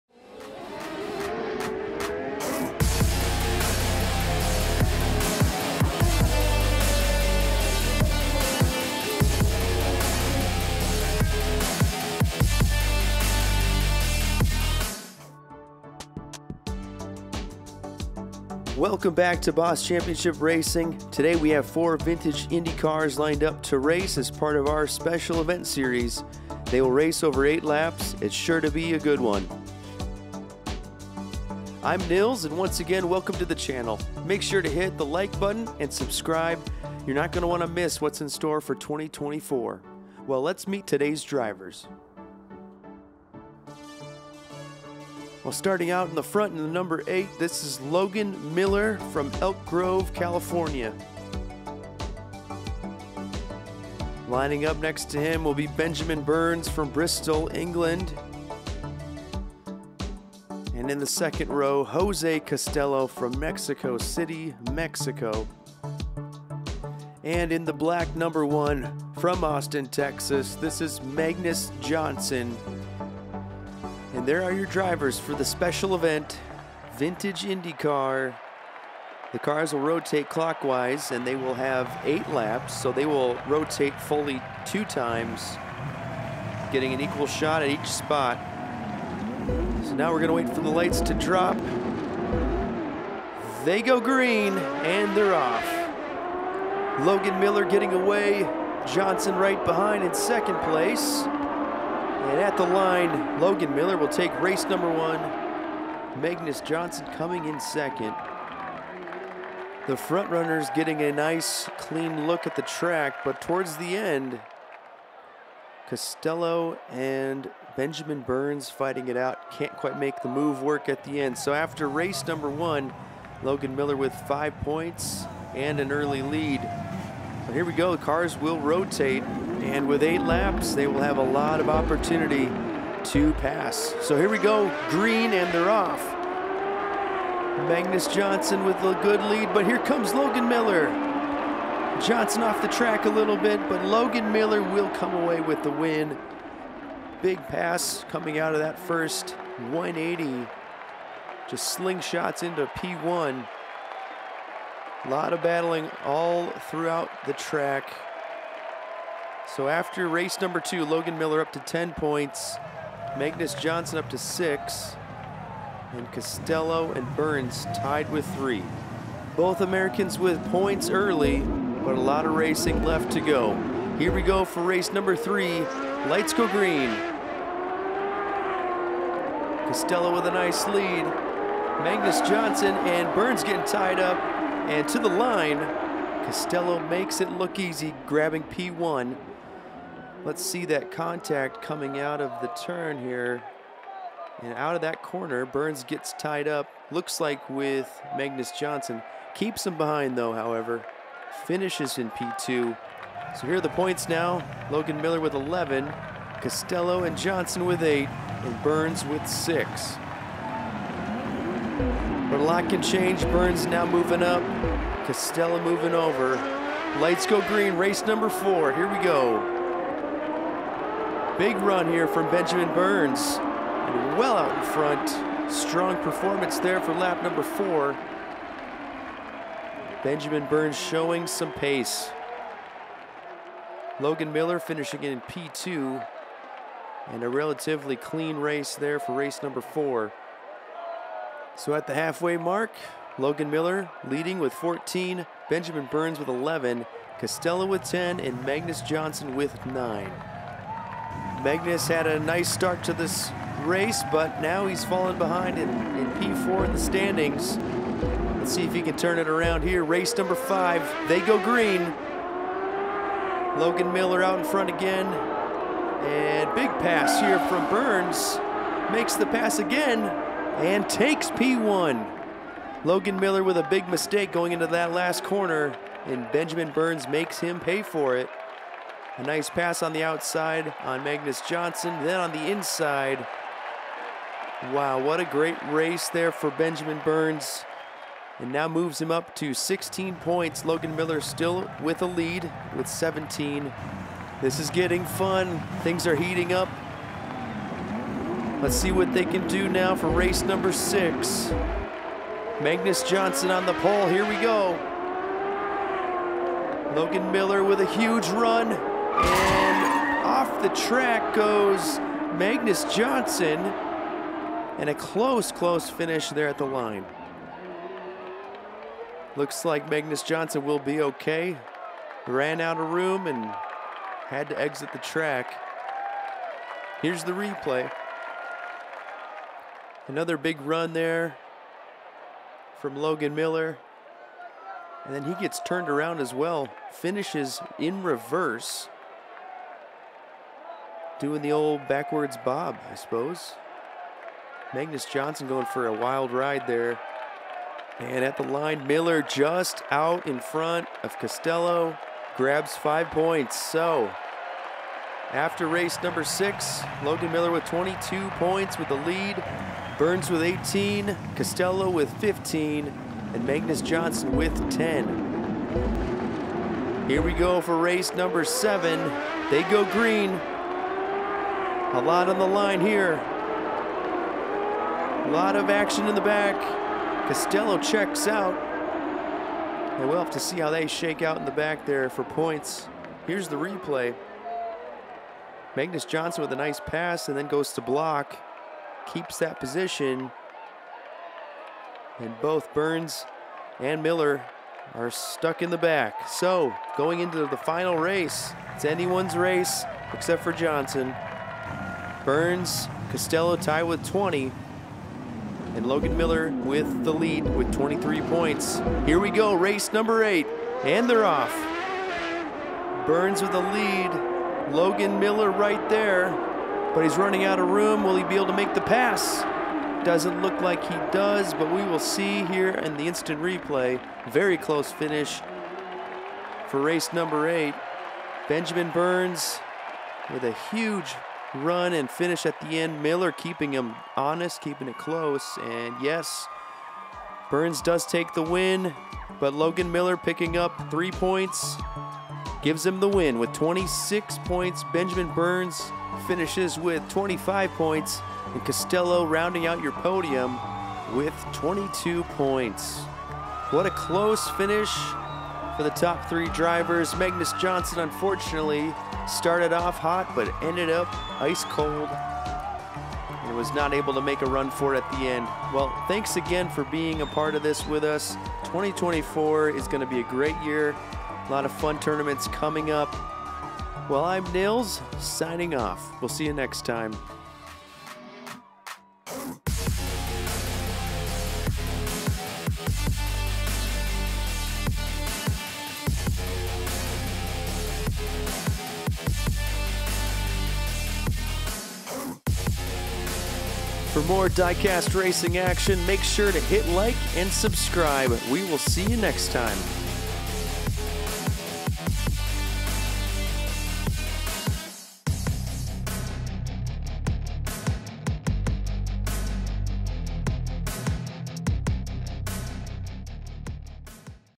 Vintage IndyCar CART Hot Wheels (Special Event) | Diecast Car Racing
Boss Championship Racing – 1/64 Diecast Car Racing

Vintage IndyCar Special Event! 8 Laps of Finesse.

*This content is intended for race car fans 13+

⚡️⚡️ Boss Championship Racing Store!